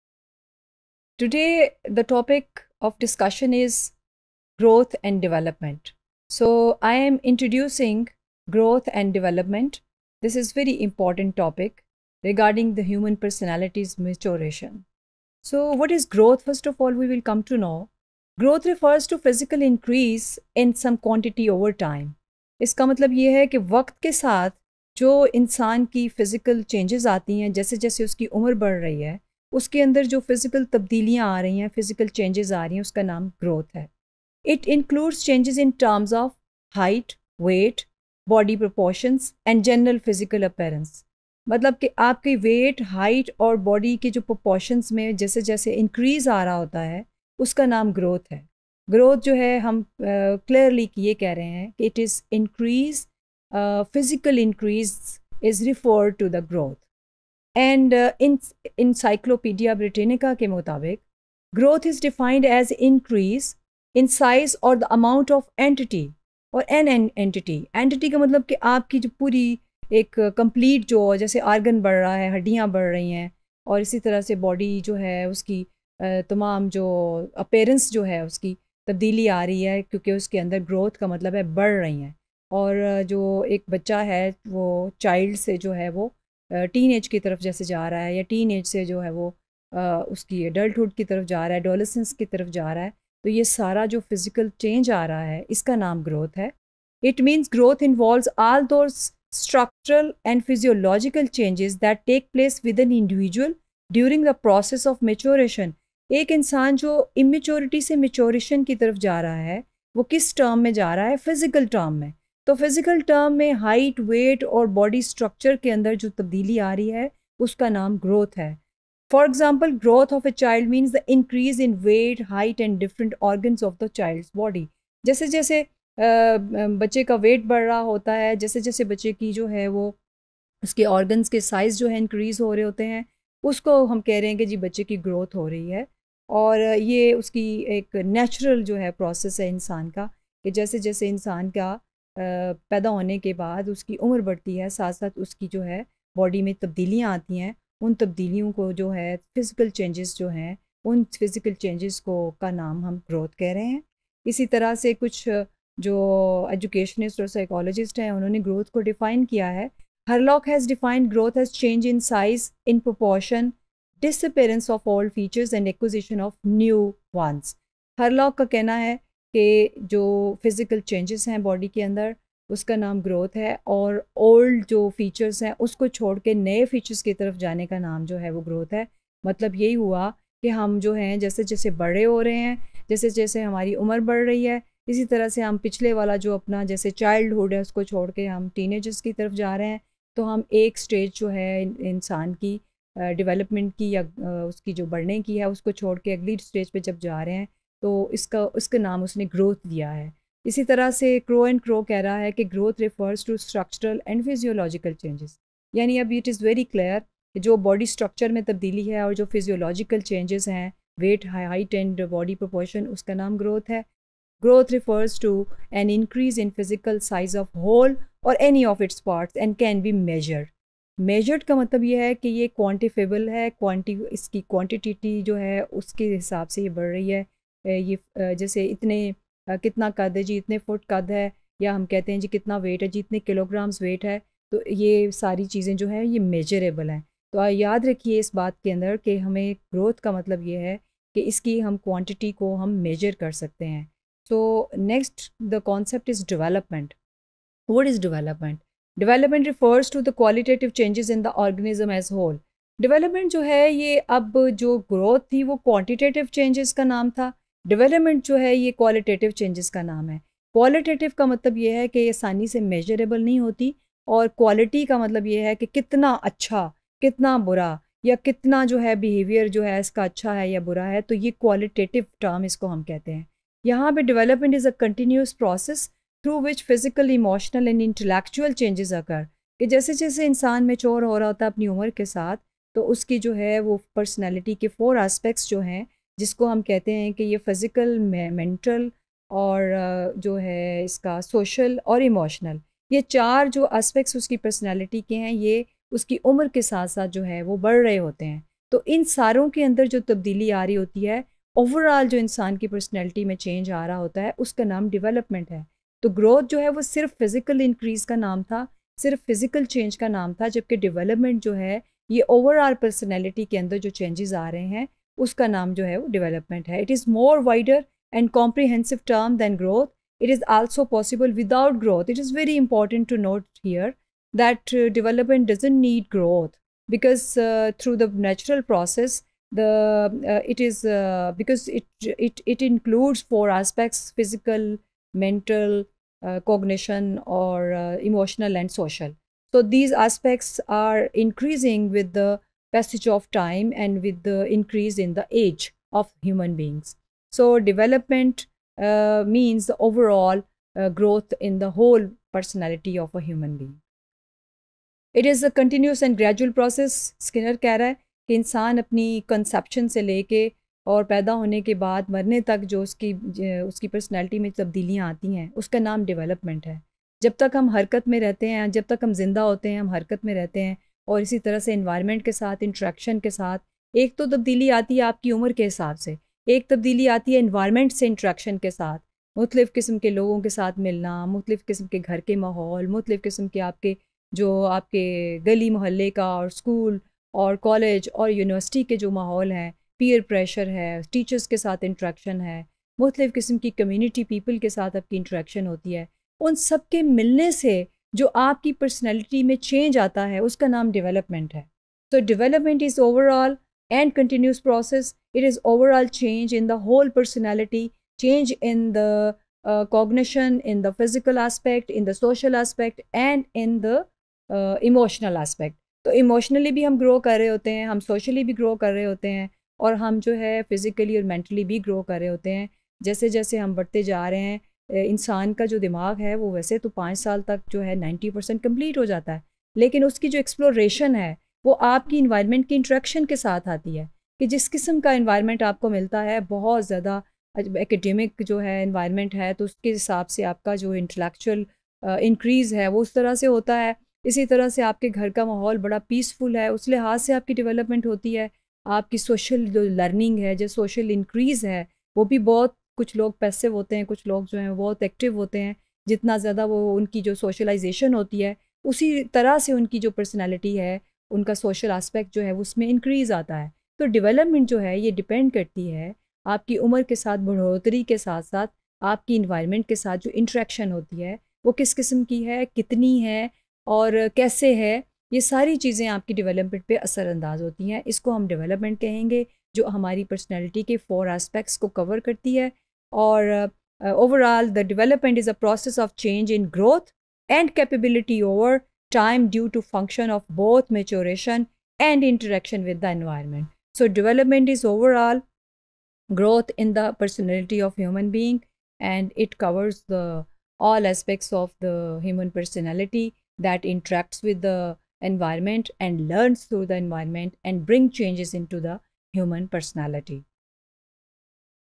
Introduction of Growth and Development | Human Development and Learning | EDU302_Topic001
Topic001 - Introduction of Growth and Development
By Dr. Sumaira Rashid